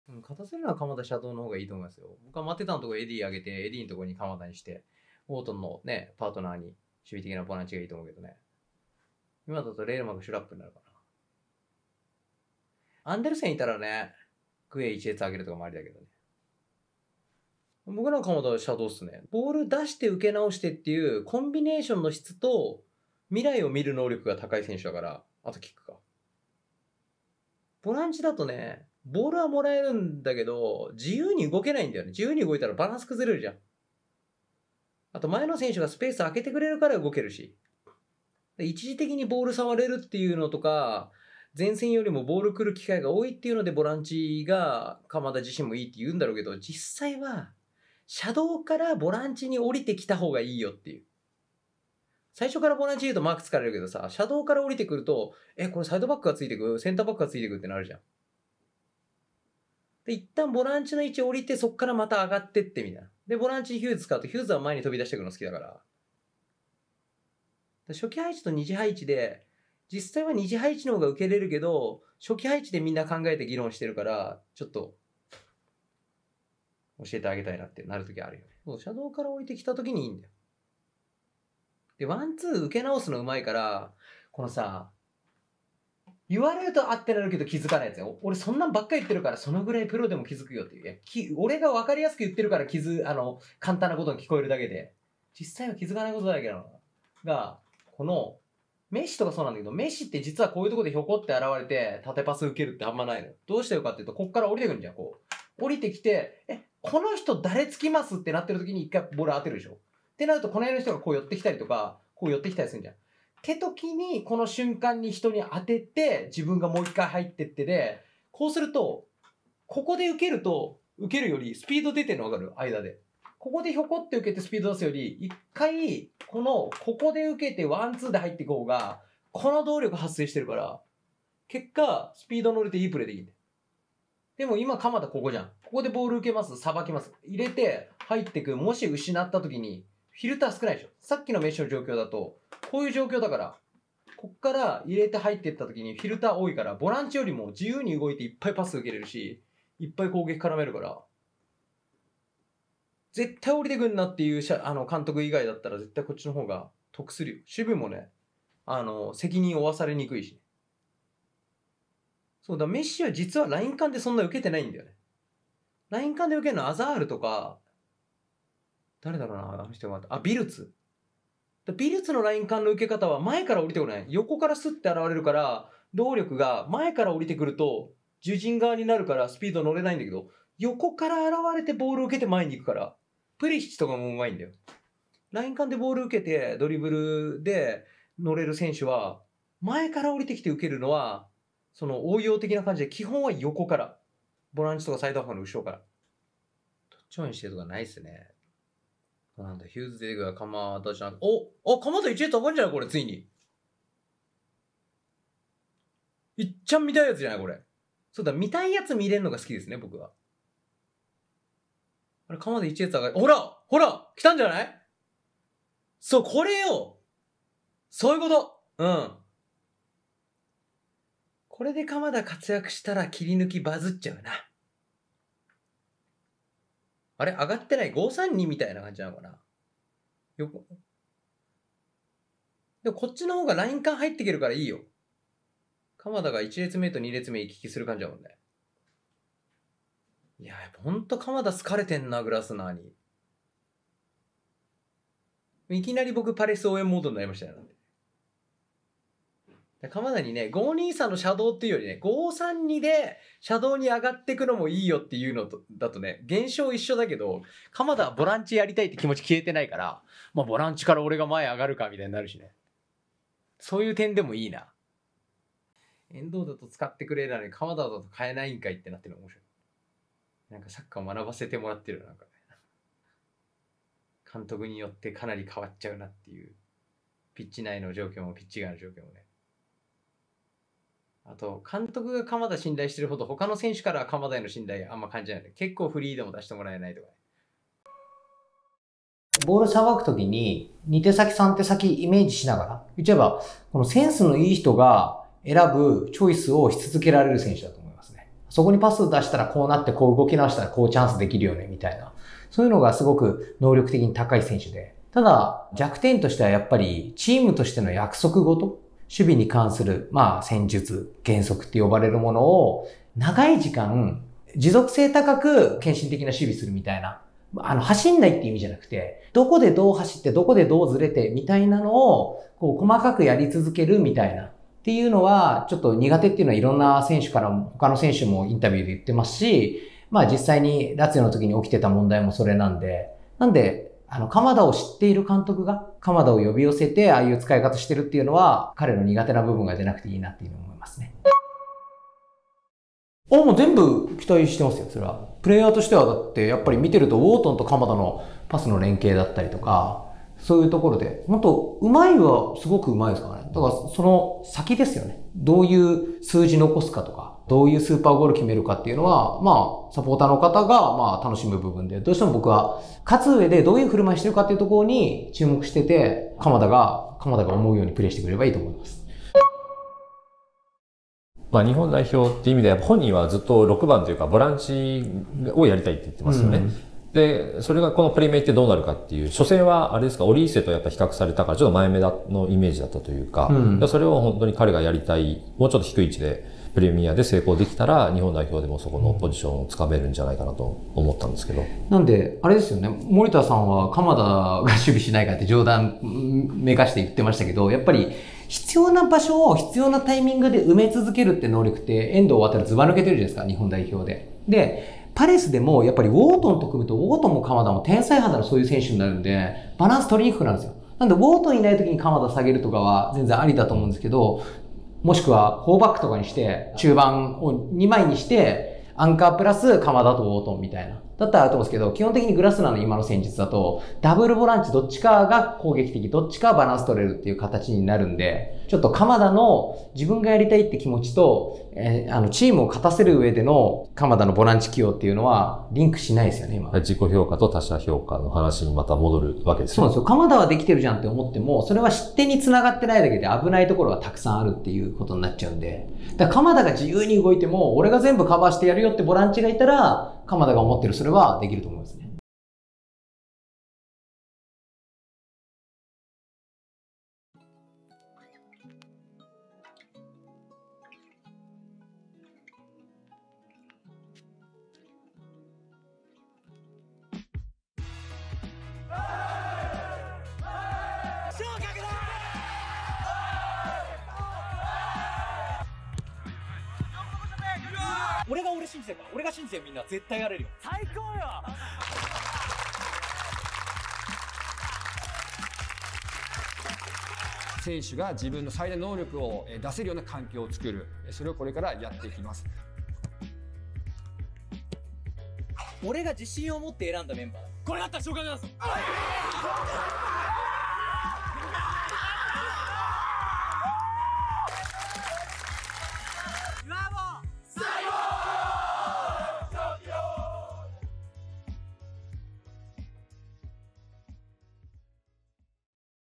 【レオザ】鎌田大地がマンチェスターユナイテッド戦でフル出場！使うべきポジションはココです。【レオザ切り抜き】
0:00 ユナイテッド戦でボランチ出場について
3:17 試合中に鎌田の役割が変わる
4:25 フル出場について
4:47 鎌田大地に合うポジション
6:11 日本代表でのポジションは？